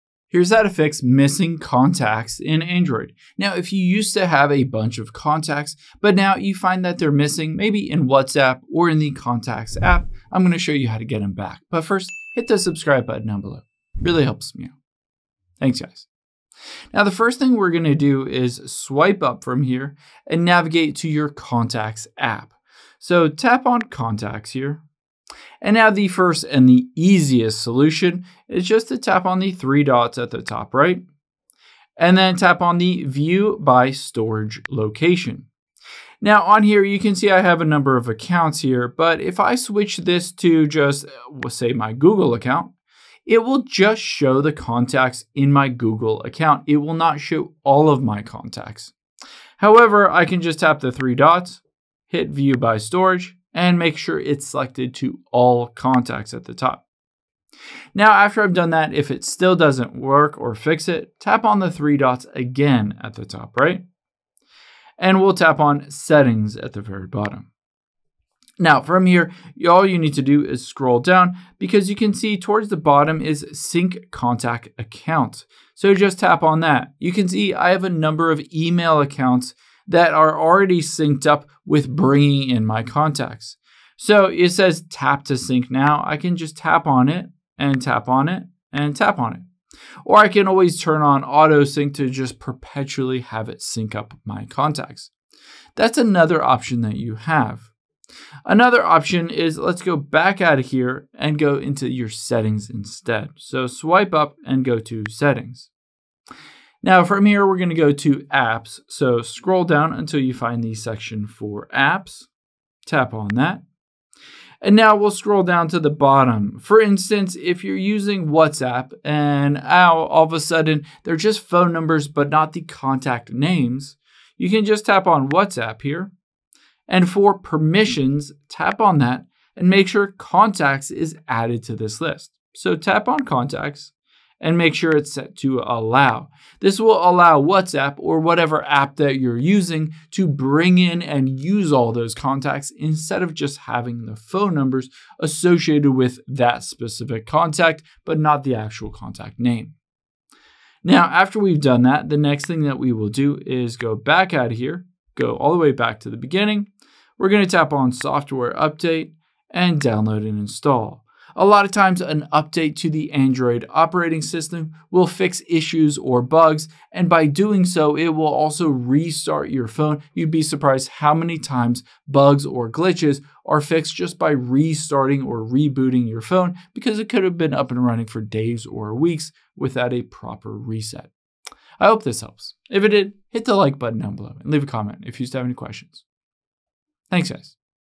How To Fix Missing Contacts On Android
Here's how to fix your Android contacts if some are missing or information in the contacts is missing.